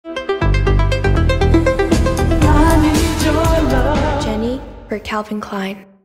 JENNIE（BLACKPINK）、Jennie for Calvin Kleinリリース-2
[Calvin Kleinは、ブランドアンバサダーであり国際的なアーティストであるグローバルアイコン、Jennie Kim／ジェニー・キムとのパートナーシップによる、限定版カプセルコレクションJennie for Calvin Kleinのリリースを発表。ジェニーのカジュアルなワードローブの中にあるエッセンシャルアイテムからヒントを得たこのコレクションは、彼女独自のスタイルとクリエイティビティが吹き込まれたアンダーウェアのセット、デニム、T シャツ、スウェット、ニットなどのアイテムで構成されており、ジェニーがキュレーションしたパステルカラーのパレットで展開される。また、ジェニー自身のハンドライティングで描かれたCalvin Kleinのシグネチャーロゴなど、パーソナルなタッチを加えたカスタムブランディングも要素に含まれている。